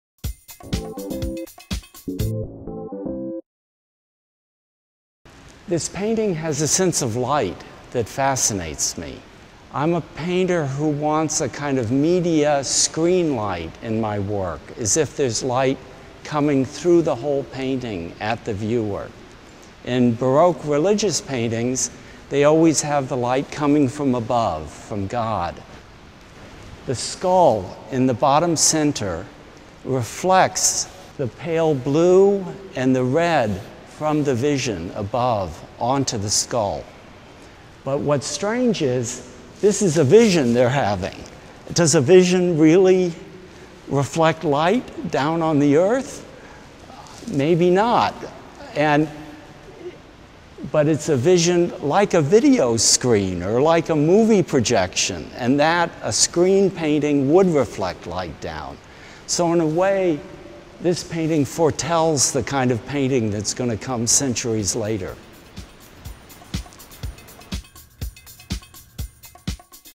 Art Inspires: David Reed on Castiglione
Art Inspires, an ongoing series of short films, invites artists to discuss works of
art from the MIA's collection that they find interesting, compelling, or
influential.

In this video, David Reed discusses Italian Baroque painter Giovanni Benedetto Castiglione's 'The Immaculate Conception with Saints Francis of Assisi and Anthony of Padua'.

David Reed (b. 1946) is famous for his sensuous abstract paintings containing swirling brushstrokes. The artist smoothes the surface of his paintings by sanding away layers of paint and glaze, giving each canvas a transparent glow. Reed draws inspiration from a variety of sources, including Abstract Expressionism and Baroque and Mannerist painting.